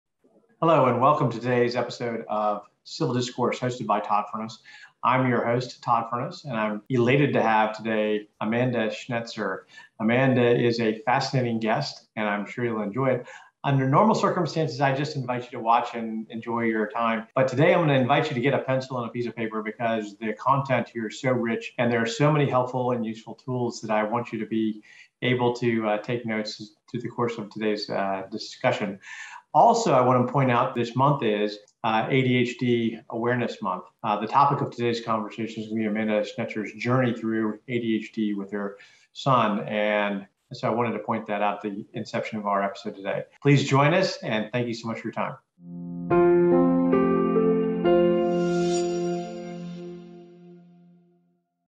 Watch Amanda Schnetzer with FirstThen Inc. on the Civil Discourse channel.
FirstThen Inc. helps transform vision into action in the business and social impact arenas.